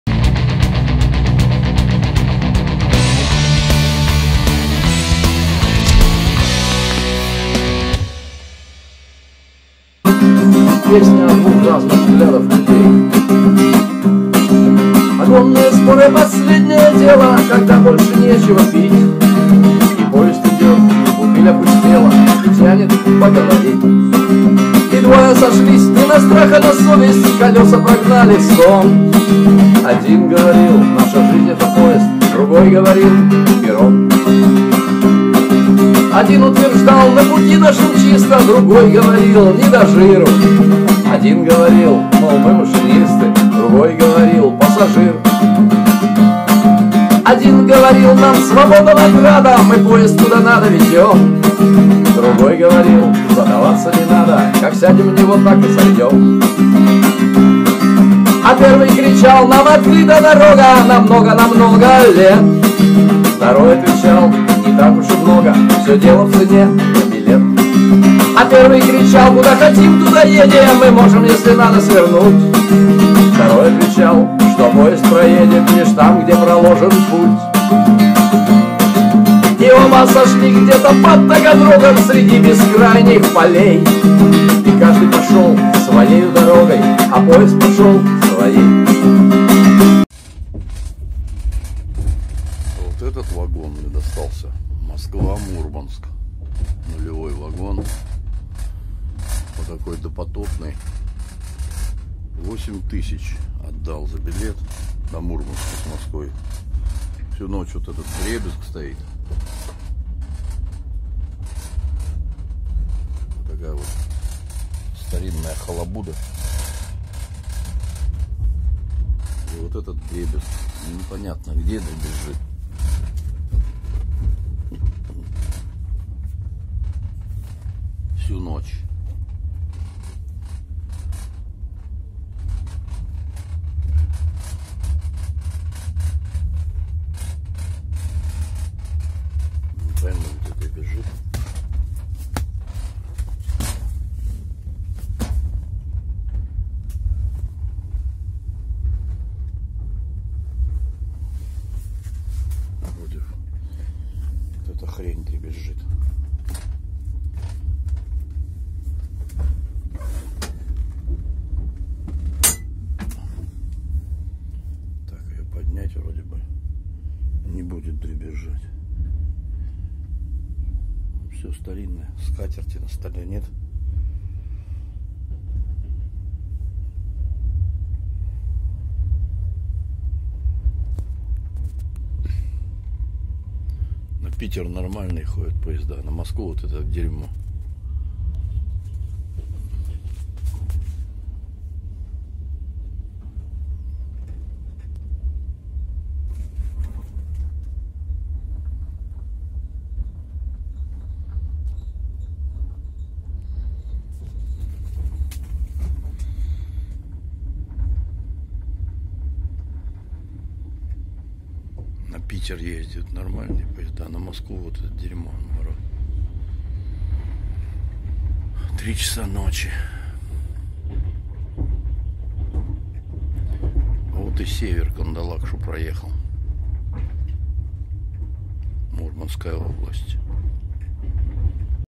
Песня о взглядах людей ,и в каком дерьме я ехал, за 8 500 рублей. ЖД-ОЧНИСЬ 21-век
И в конце показываю , в каком дерьмовом купе,за восемь тысячь птьсот рублей я проехал от Москвы до Мурманска.